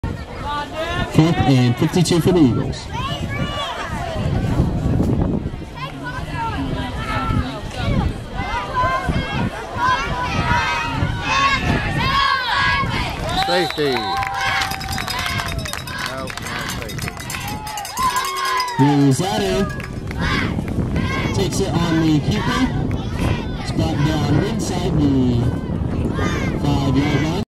Blaise Jones #20 gets a sack on 4th down for a 7 yd loss - Elwood 8th Panthers vs Frankton 9-05-2023
Blaise Jones #20 gets a sack on Frankton's 4th down for an 7 yard loss in the Elwood 8th Grade Panthers 27-0 shutout win over the Frankton 8th Grade Eagles. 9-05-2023